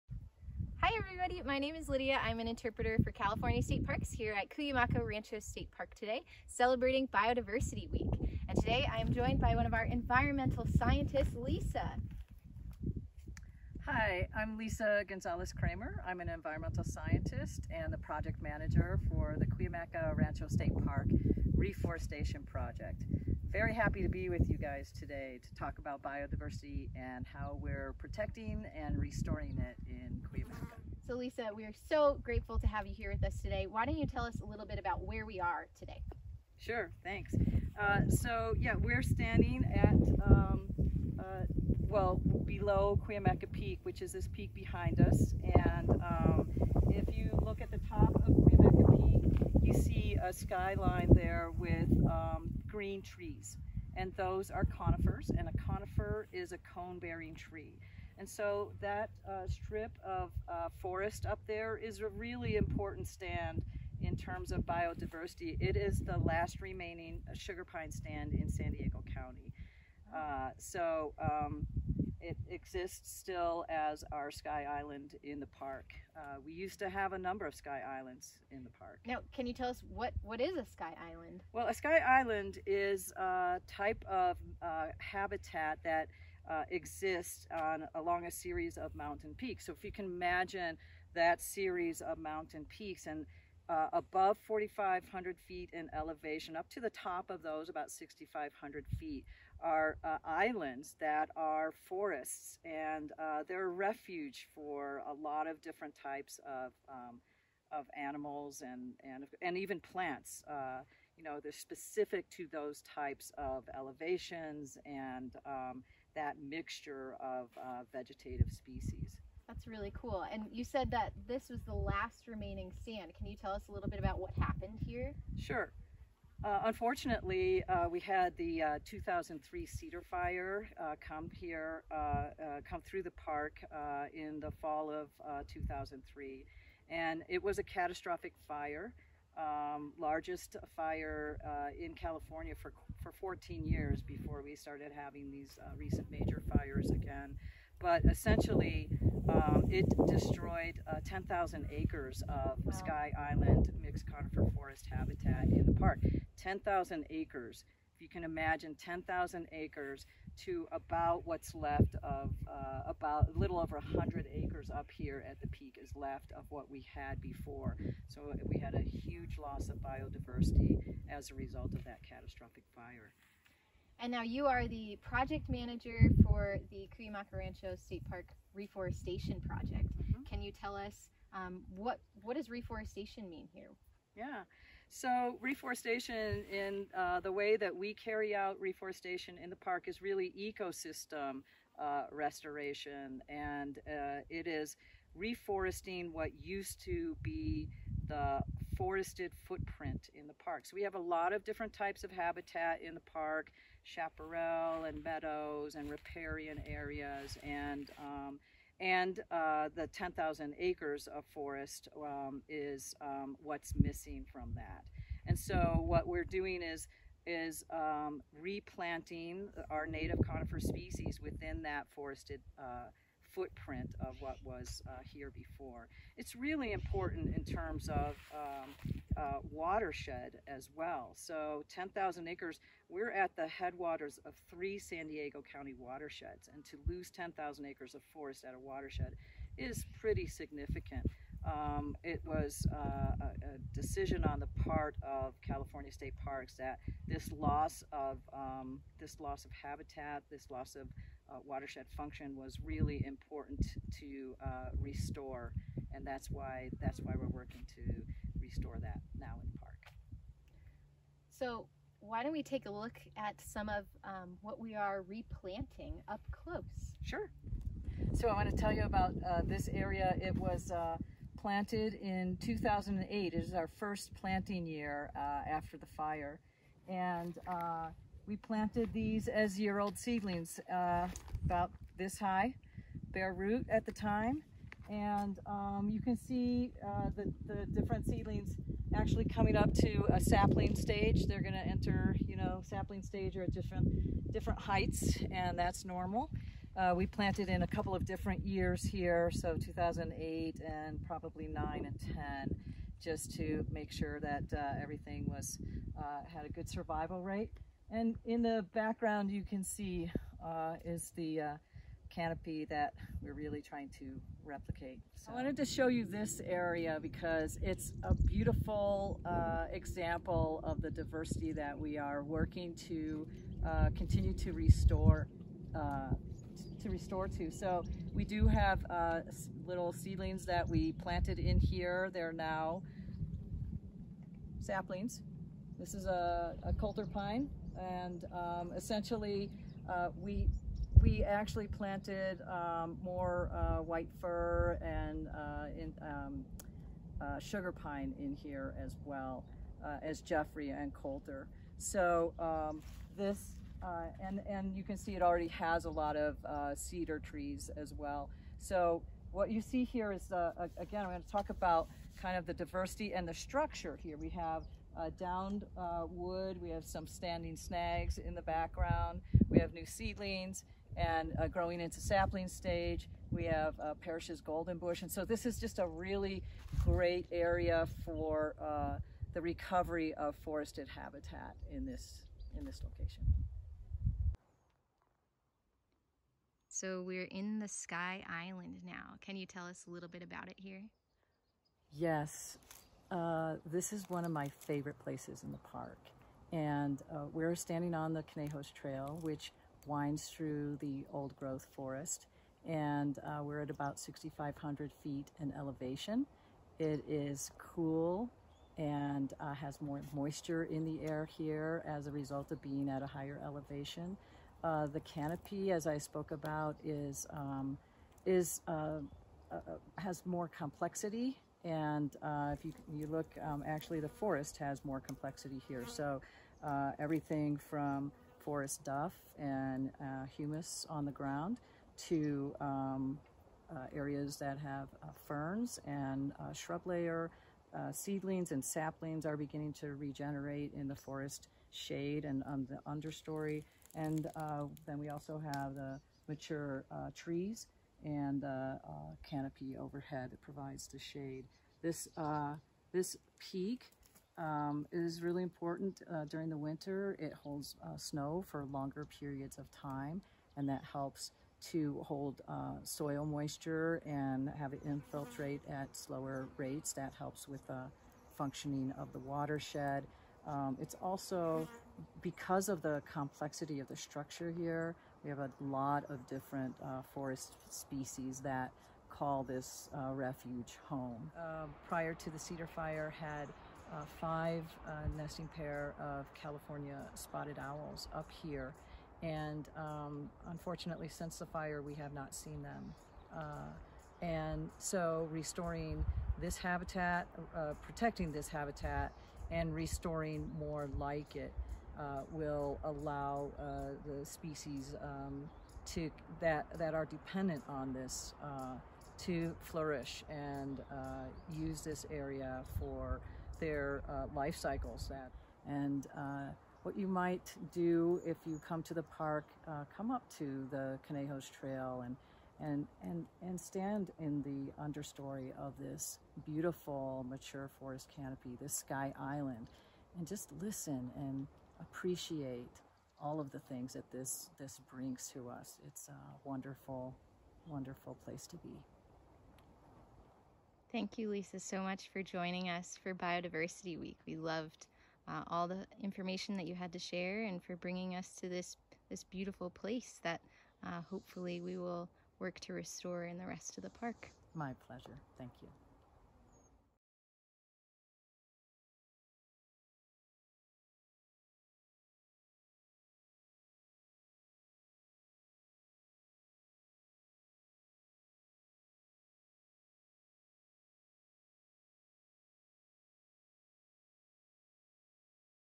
Cuyamaca Rancho State Park Restoration Project Interview
This video interview was created for Biodiversity week 2020. State Park Interpreter Lydia learns from Environmental Scientist Lisa about the restoration project and progress happening in Cuyamaca.